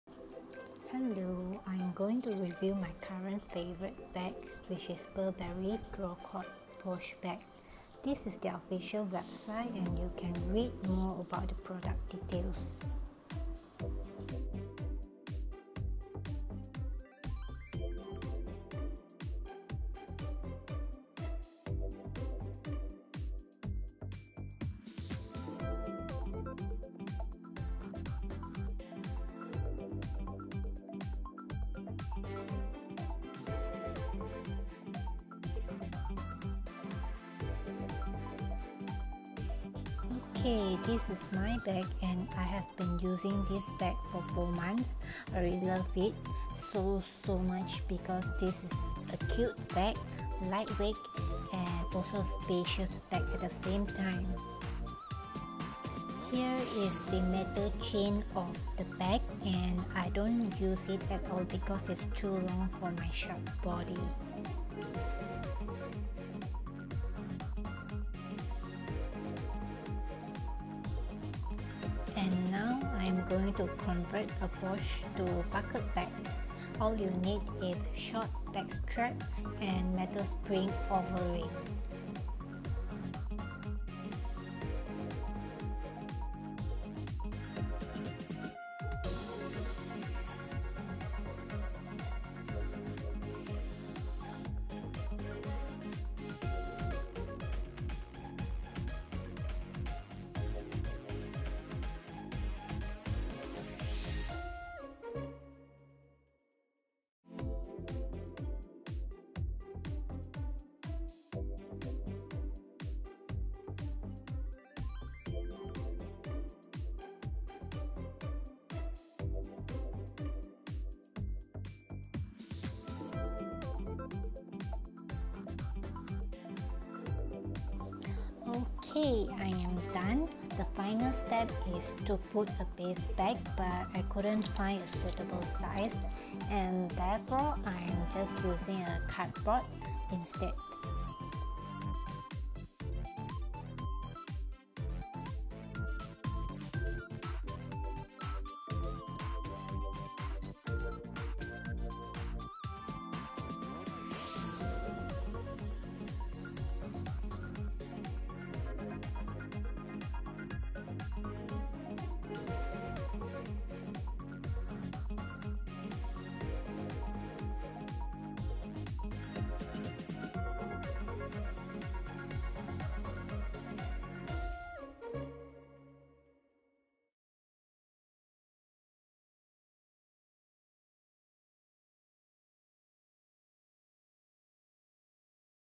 Burberry Print ECONYL® Drawcord Pouch Review
Logo Print ECONYL® Drawcord Pouch
Color: Black

PRODUCT DETAILS

A drawcord pouch in logo-tagged ECONYL® – a sustainable nylon yarn made from regenerated fishing nets, fabric scraps and industrial plastic. Use the zip pocket to secure smaller essentials.
18 x 12.5 x 19.5cm/7.1 x 4.9 x 7.7in
Outer: ECONYL® (100% polyamide)
Trim: 100% calf leather
Lining: 100% polyamide
One exterior zip pocket
Drawcord closure
Hand-painted edges
Printed Burberry lettering at front
Imported
We are designing products using recycled materials, led by our commitment to drive positive change throughout our supply chain. This product contains recycled material.
Item 80321881